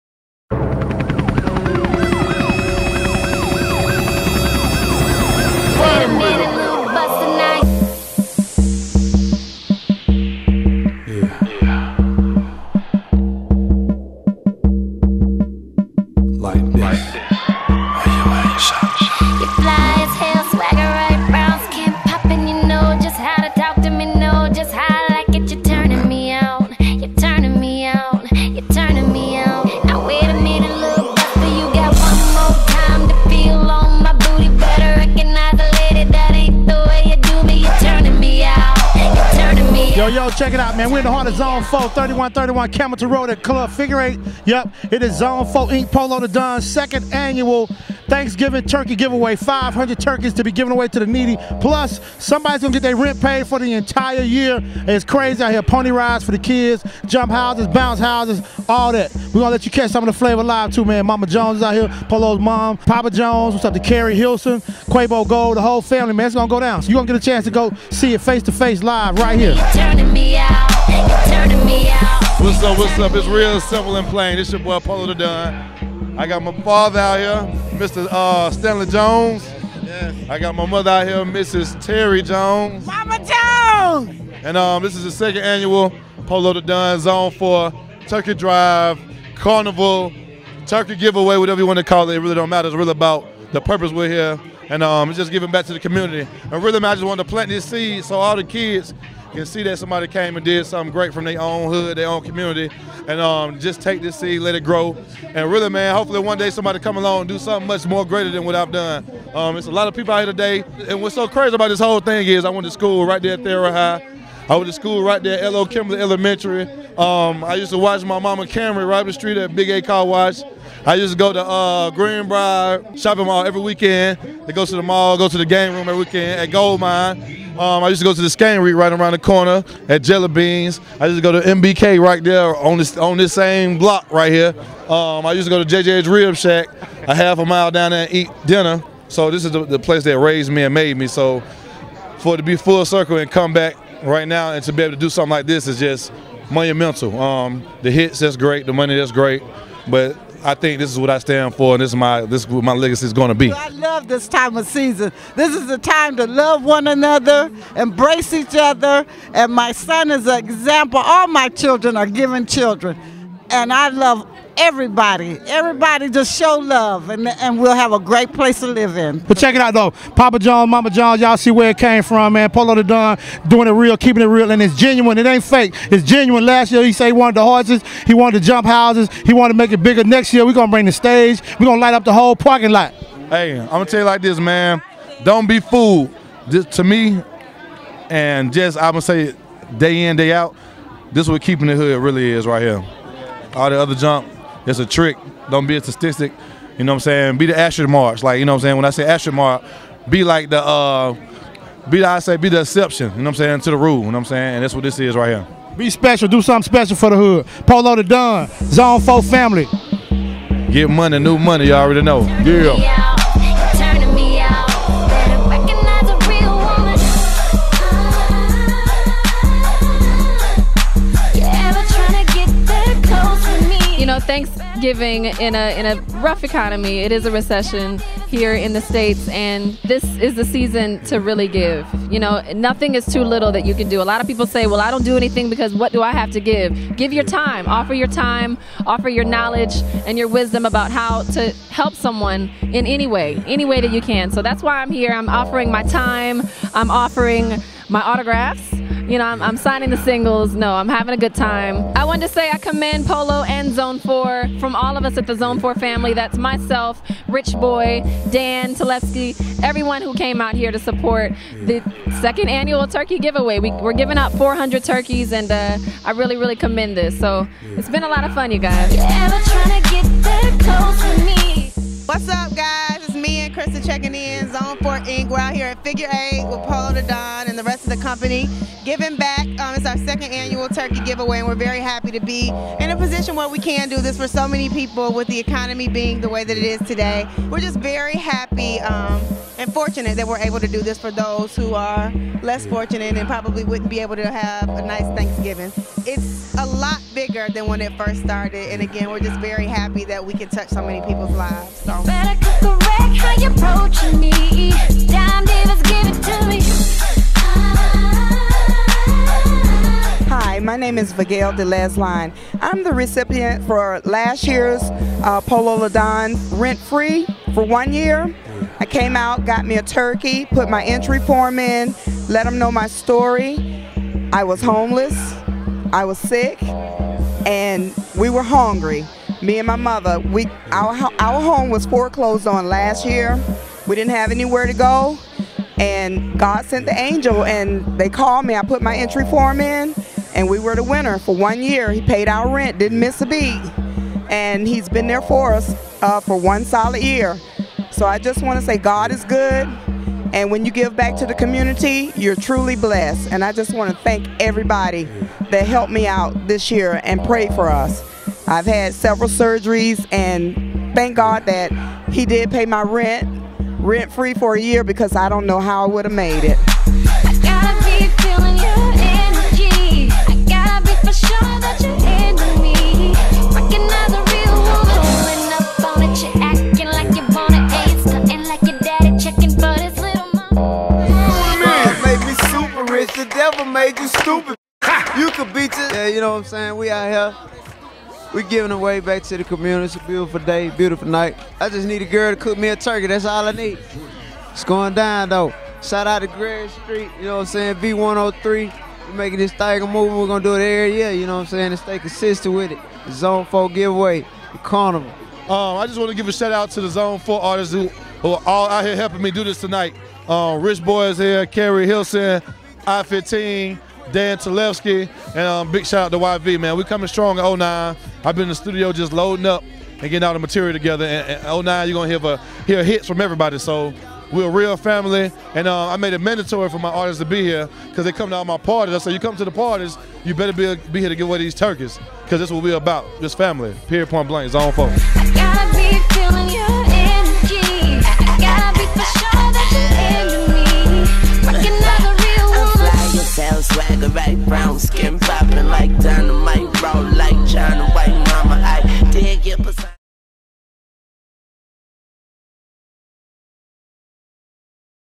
Polow da Don Thanksgiving Carnival
Polow da Don and Zone 4 Inc. puts it DOWN every Thanksgiving here in Atlanta Ga. D. Jones Productions is a product of Zone 4 and we were there. Share the experience. Zone 4 Inc. .Get Money! New Money!